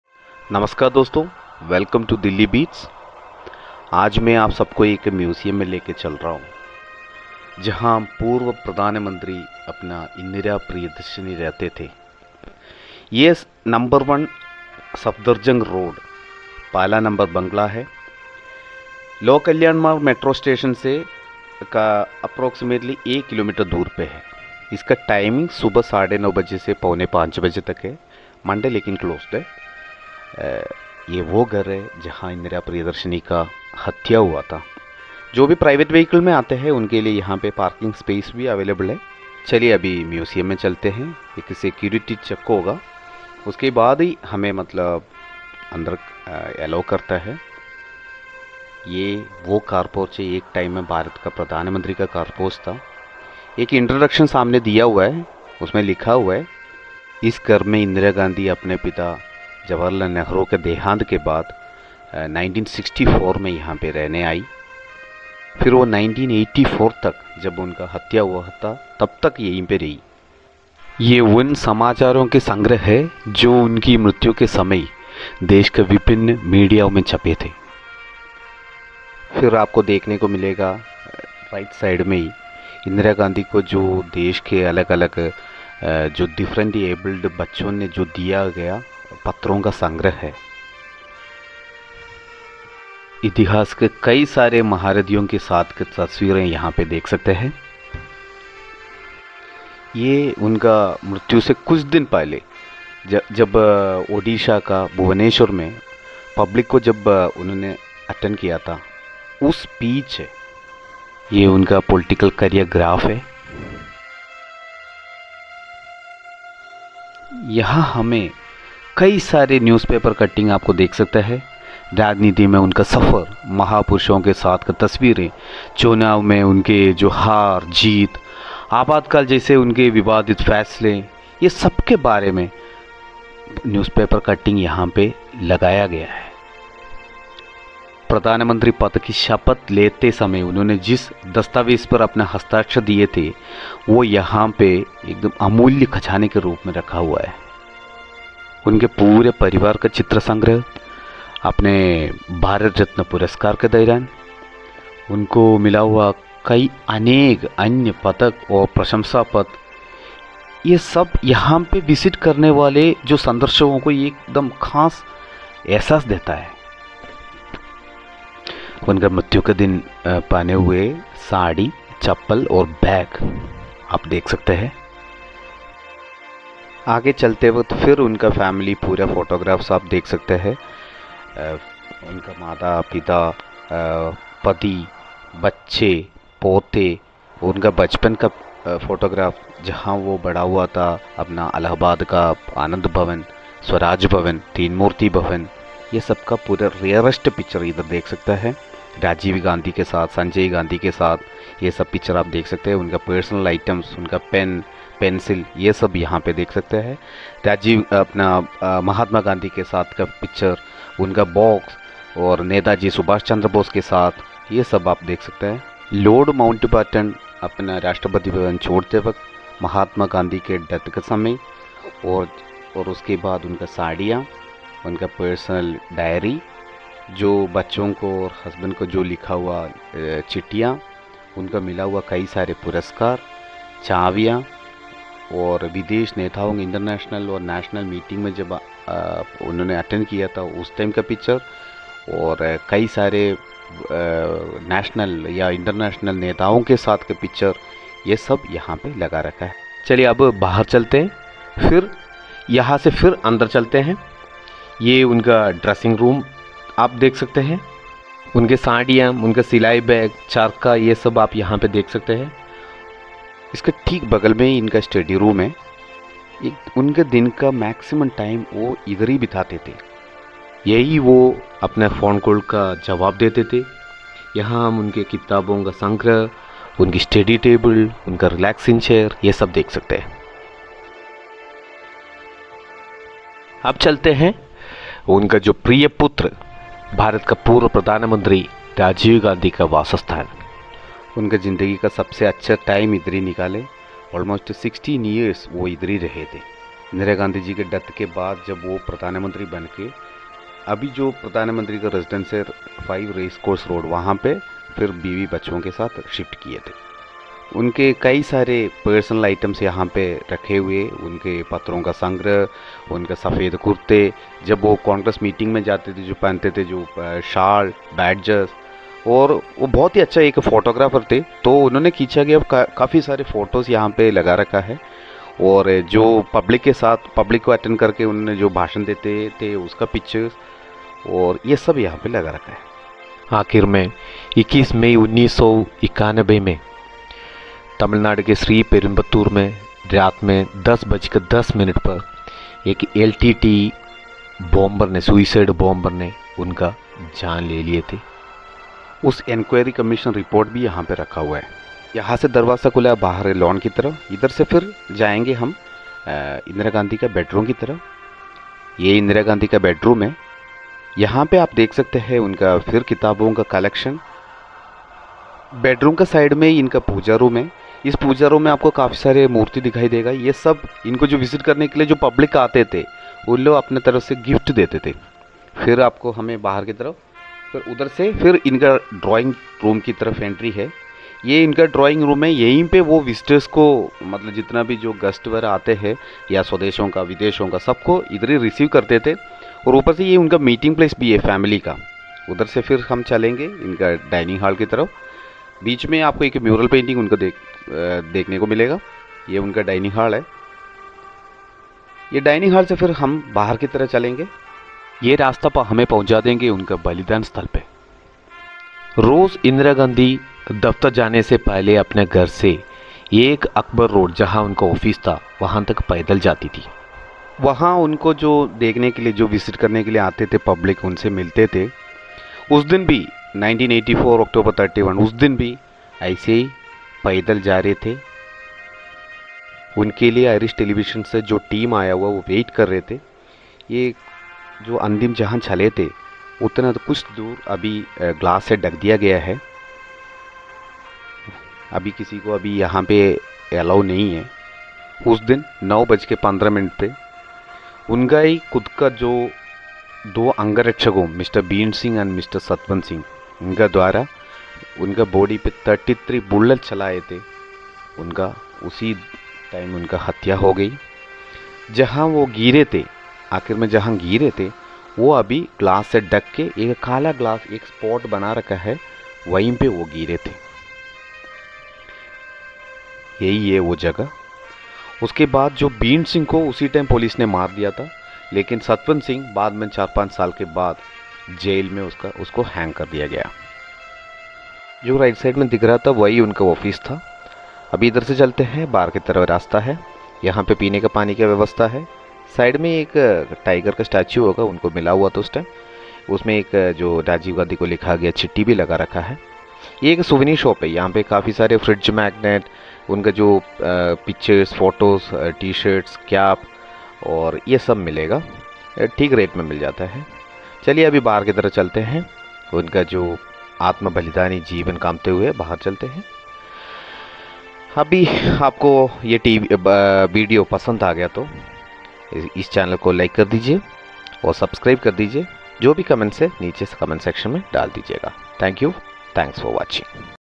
INDIRA GANDHI MEMORIAL MUSEUM, New Delhi/ Rajiv Gandhi'S Residence /A Complete Guide inside Museum
A Complete Guide into the Museum inside the Museum.
Complete details about
Location
Timing
Parking
Guest Room
Her Drawing Room
Dining Room
Bed Room
Rajiv Section
Last Remains of Indira & Rajiv
Bharat Ratna puraskar of Rajiv & Indira
Rare & Rarest photos of Both& their families
Martyrdom Place
Old PMO OFfice
Souvenir Shop
Exit